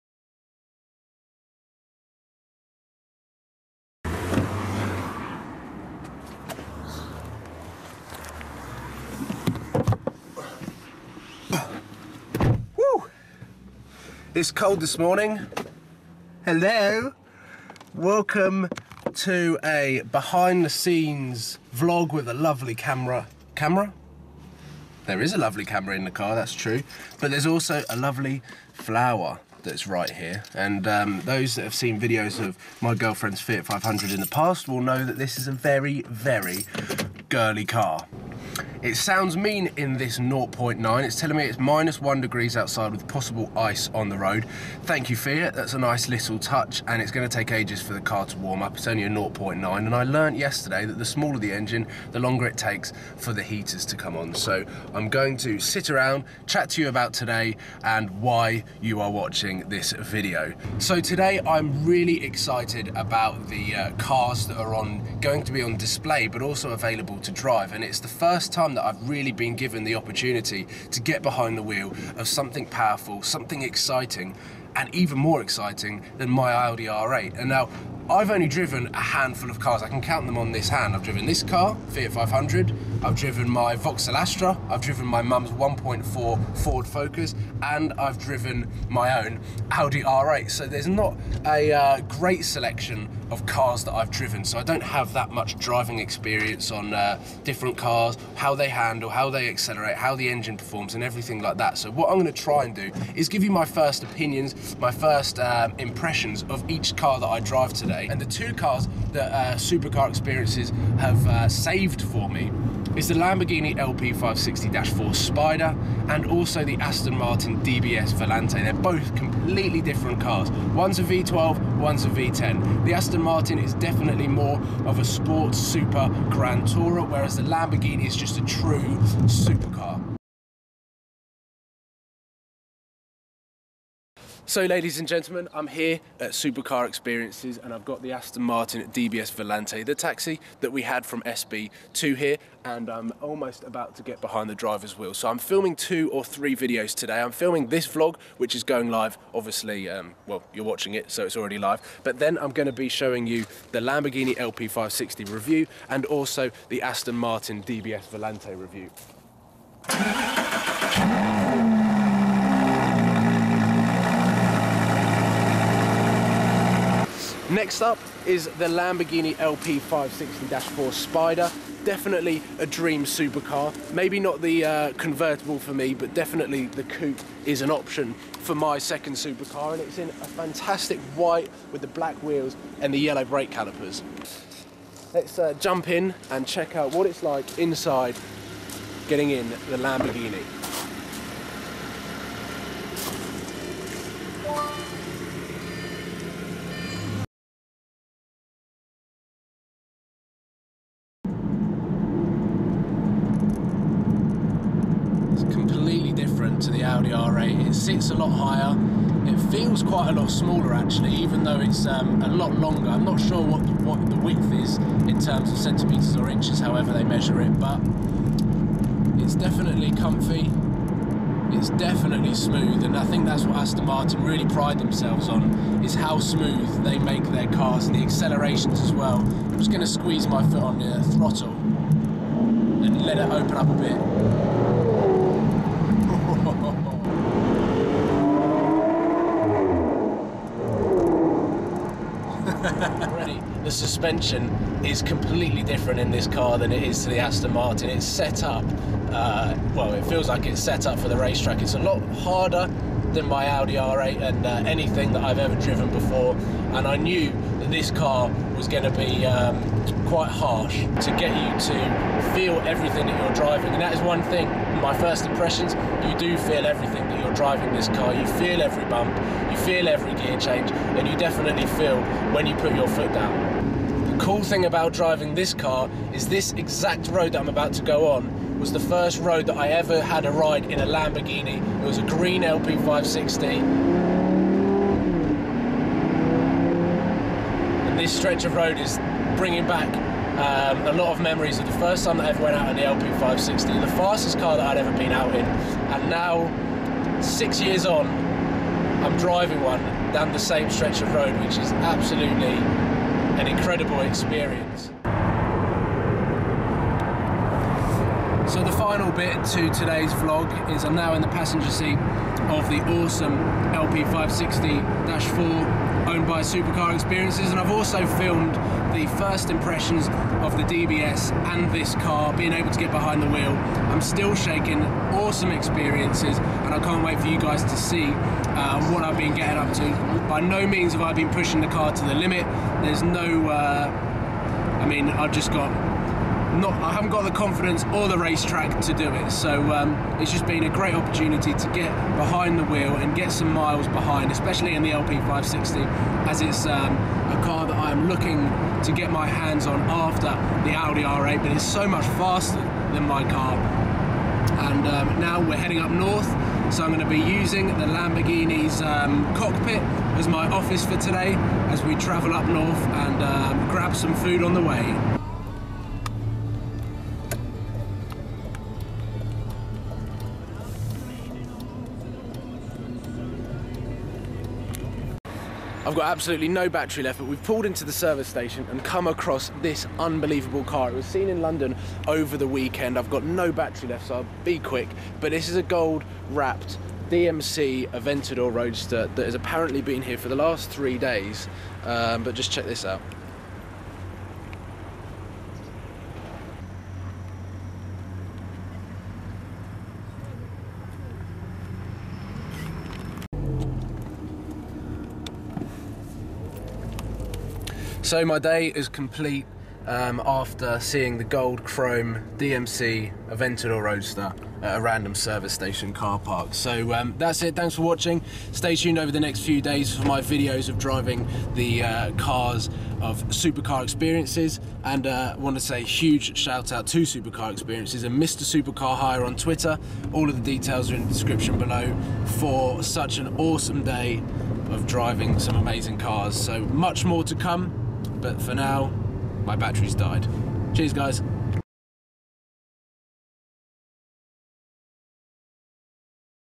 My Supercar Experience: Behind the Scenes Vlog
Huge thanks to @MrSupercarHire (Twitter) for the experience! Supercar Experience have given me exclusive access to a selection of their fleet!